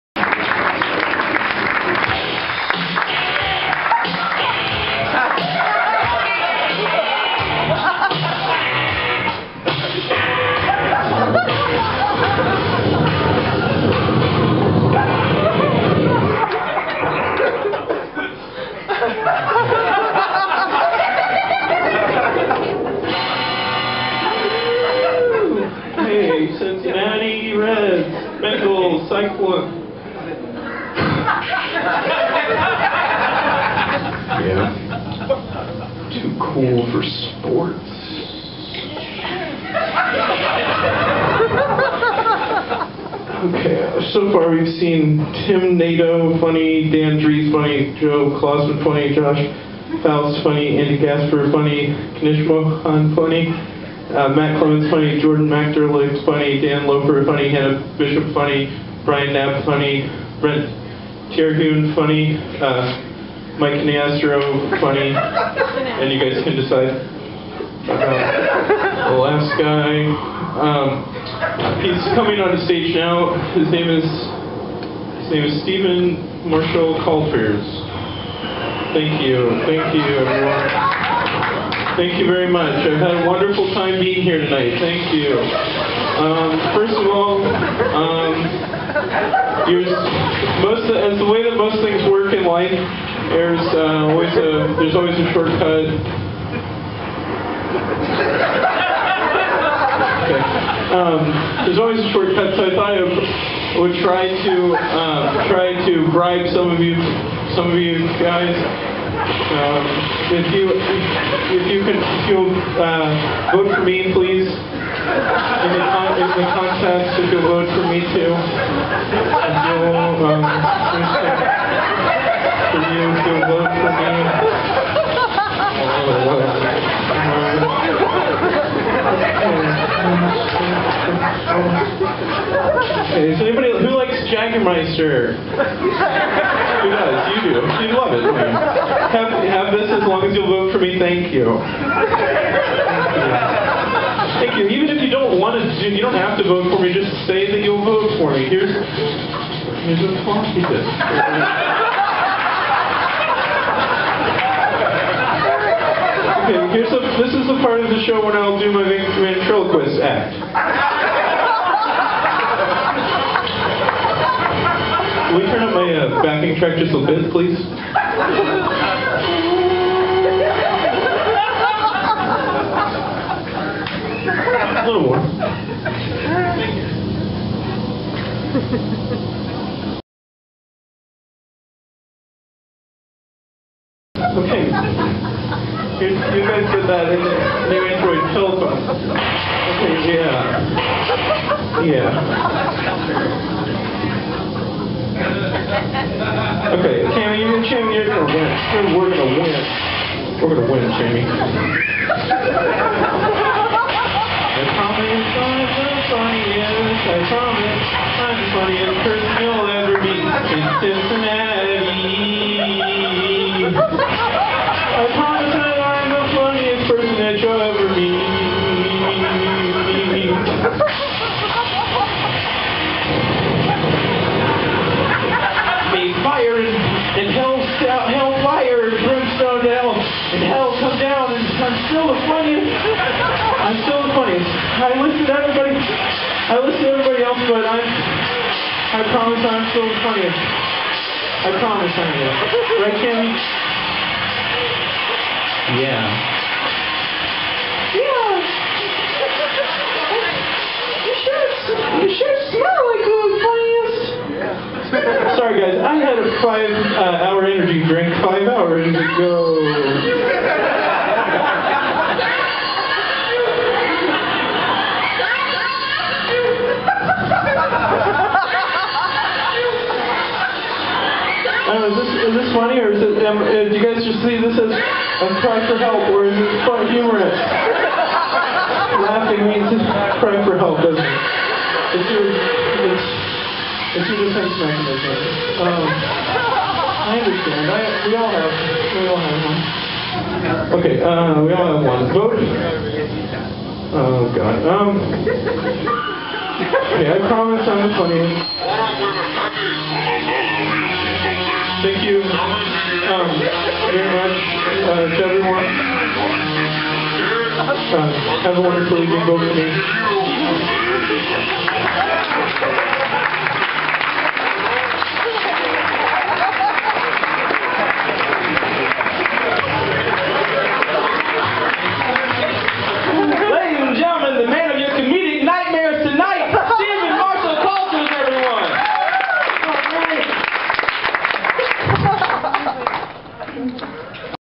Ste v v v en Mar r r shall Cal l l ters
Fatty Steven Marshall Calters is performing and attempting to try to win "Go Bananas Funniest Person In Cincinnati Contest " - he didnt win.

6/6/2012